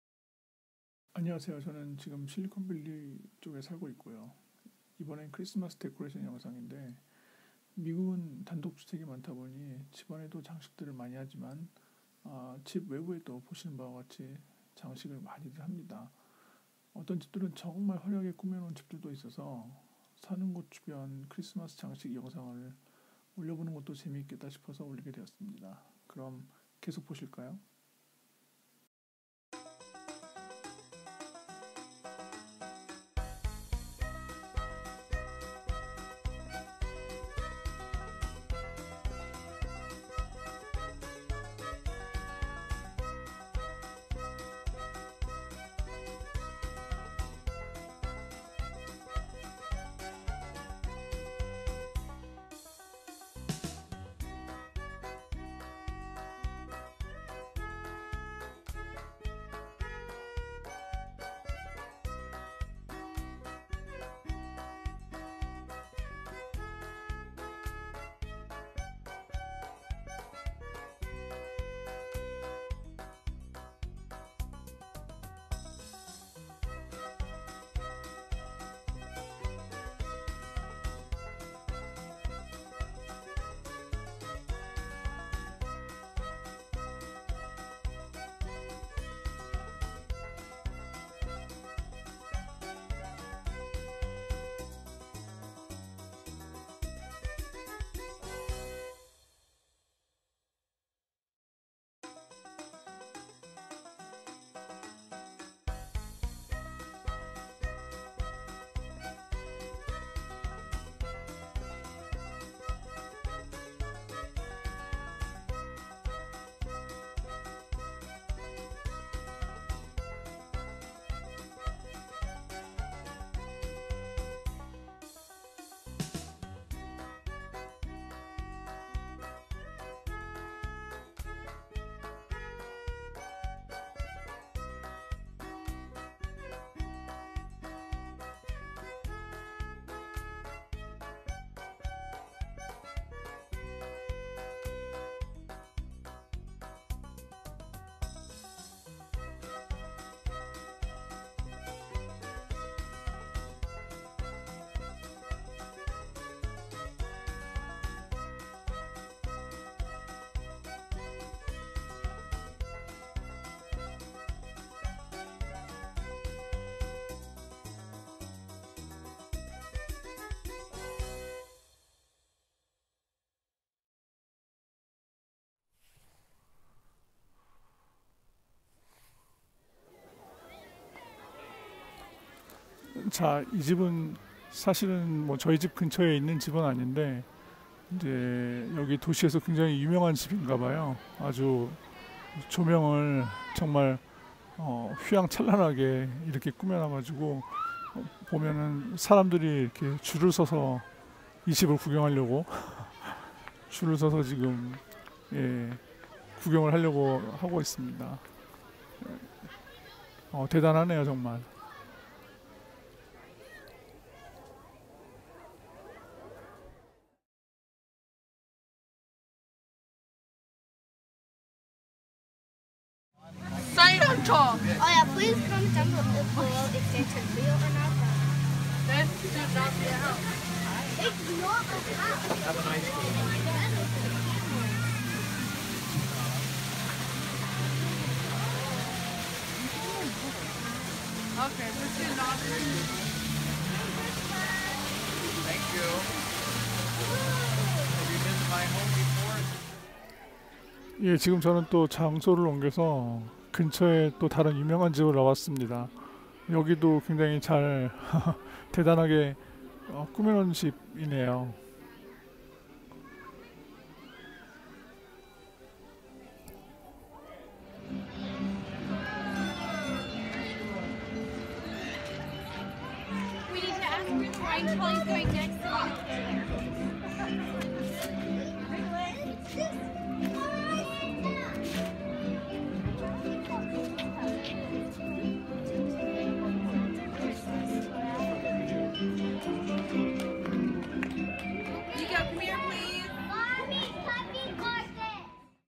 미국 집들의 크리스마스 데코레이션
미국에서, 크리스마스 즈음에 이런 집들을 구경하고 싶으시면 구글 맵에서 'Christmas lighting home' 검색하시면 주변에 잘 꾸며 놓은 집들이 나옵니다.